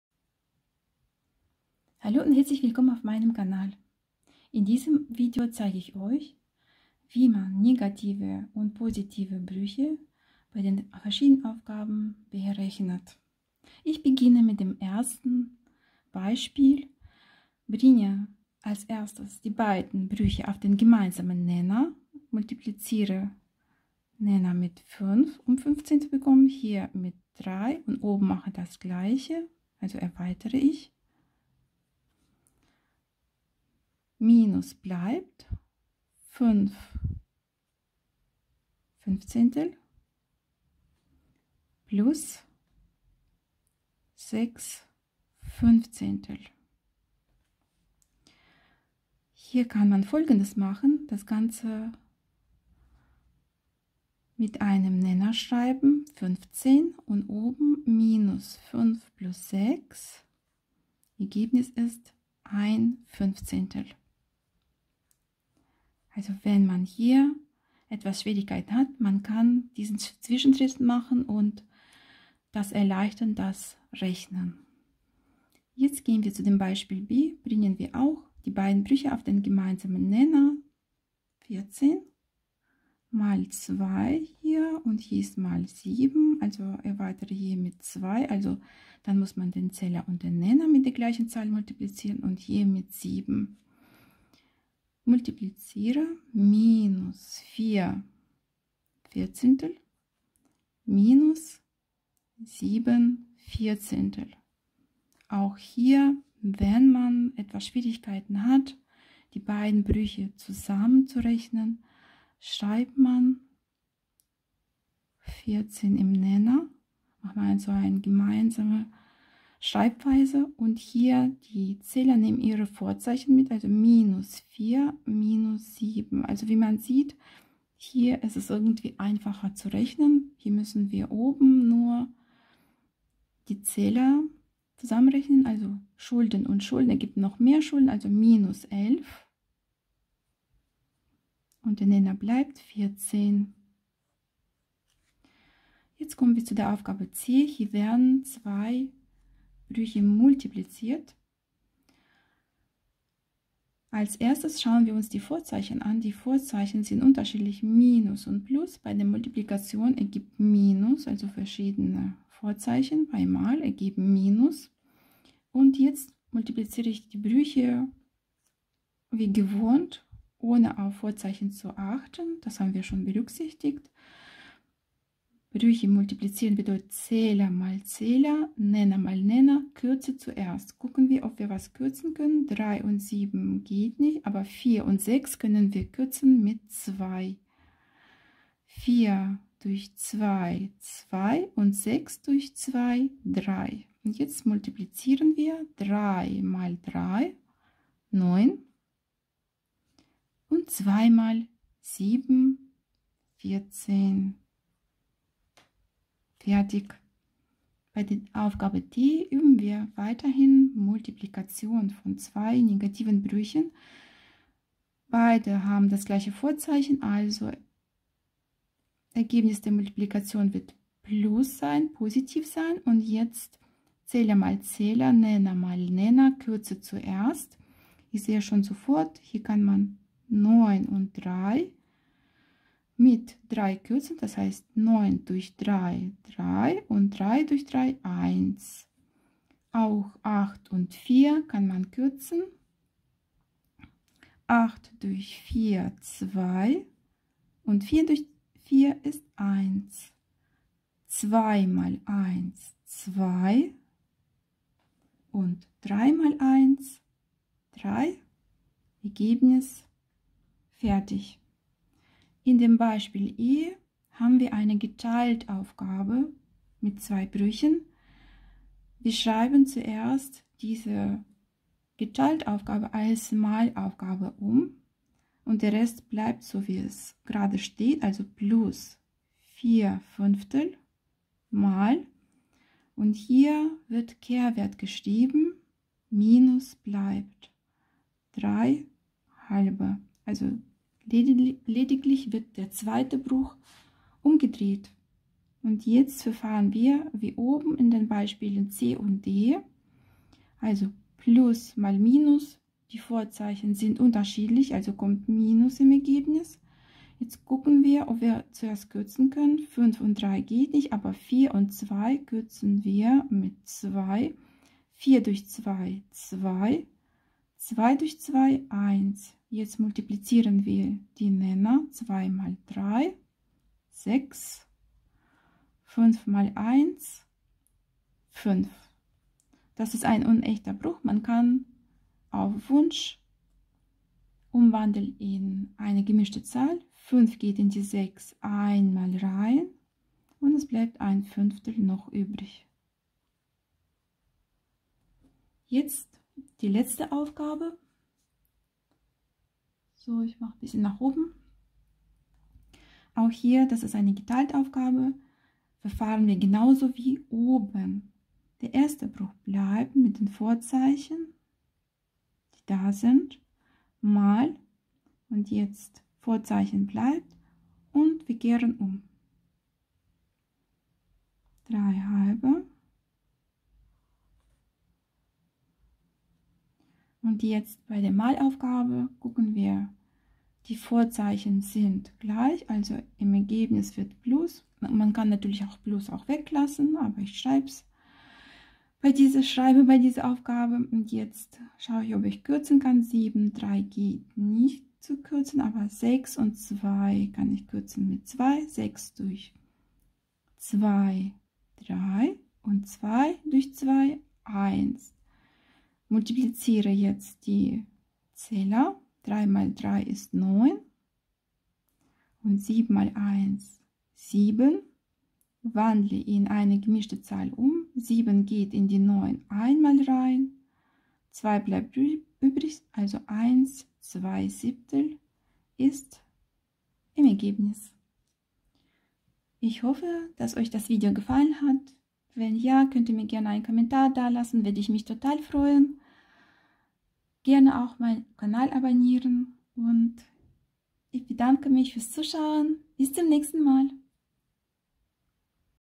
NEGATIVE und POSITIVE BRÜCHE – Rationale Zahlen addieren, subtrahieren, multiplizieren, dividieren
NEGATIVE und POSITIVE BRÜCHE – Rationale Zahlen addieren, subtrahieren, multiplizieren und dividieren
In diesem Video zeige ich euch, wie man positive und negative Brüche addiert, subtrahiert, multipliziert und dividiert. Da die Brüche verschiedene Vorzeichen haben, kann es die Berechnung erschweren. Ich hoffe, dass meine Tipps und die Erklärung hilft, solche Aufgaben problemlos zu lösen.

00:00 Begrüßung
00:15 Beispiel (a)
01:15 Beispiel (b)
02:28 Beispiel (c)
03:28 Beispiel (d)
04:22 Beispiel (e)
05:52 Beispiel (f)
07:09 Abschluss